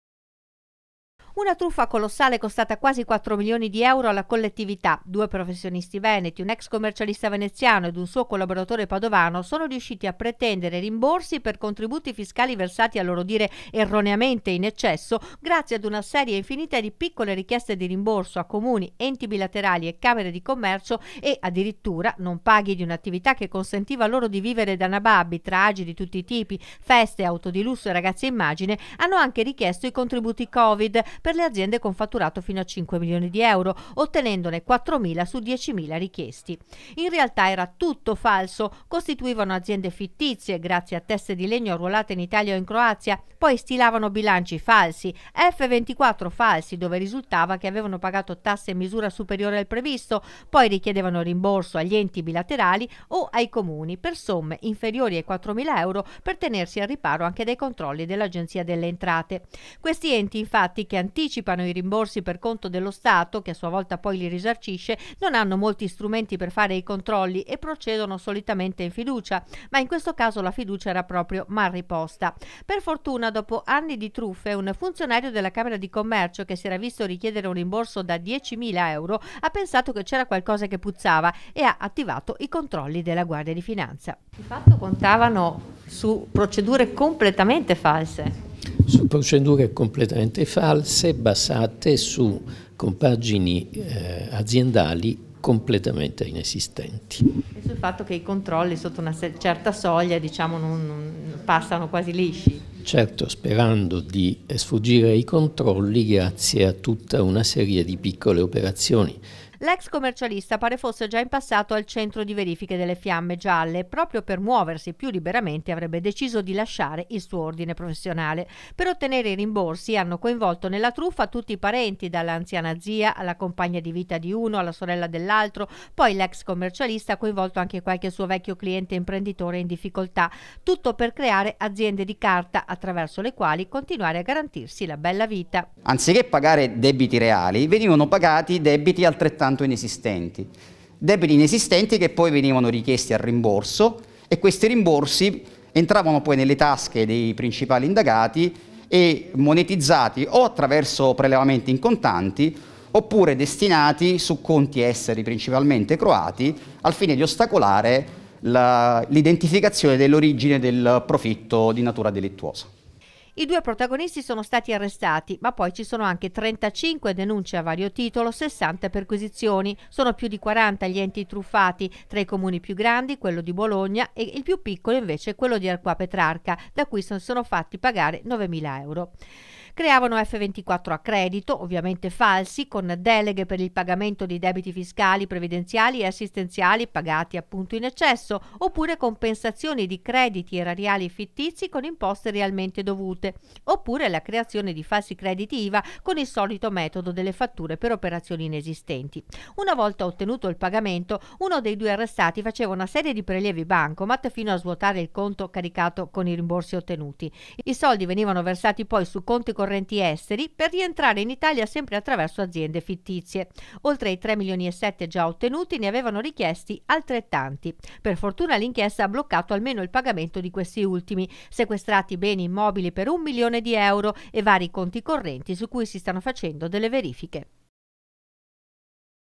TRUFFA DEI FALSI RIMBORSI DELLE TASSE
Due arrestati nell'operazione condotta da Guardia di Finanza e Procura di Padova in otto regioni italiane. Scopera una truffa colossale, basata sulla  richiesta di falsi rimborsi fiscali, ma anche di contributi per il Covid-19.